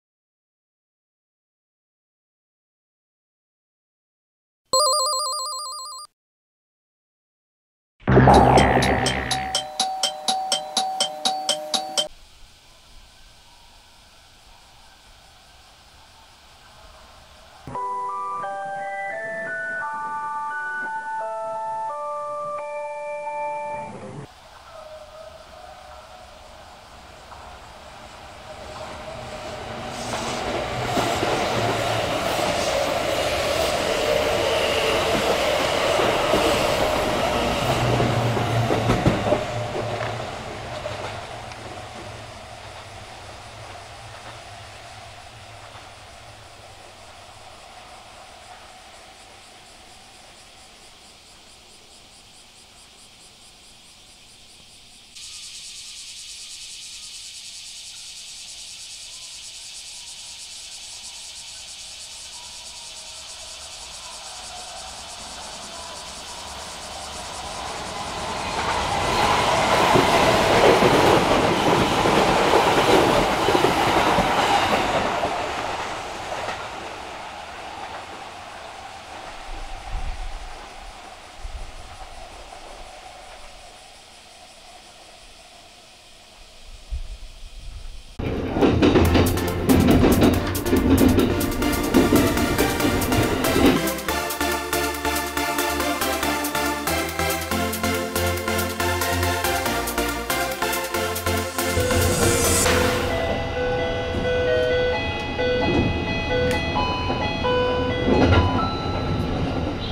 【瓢箪山変電所】特急車両の回送を撮る
最近早く目が覚めるもんで、少し遠出しました（っても自転車で１０分）
ここは好きなんですが「暑さ」には注意が必要ですね
それと列車の来る方向が解りづらい、近鉄アプリ情報とズレがあります。やはり、速達列車が近隣駅に停車しないことが原因と思います。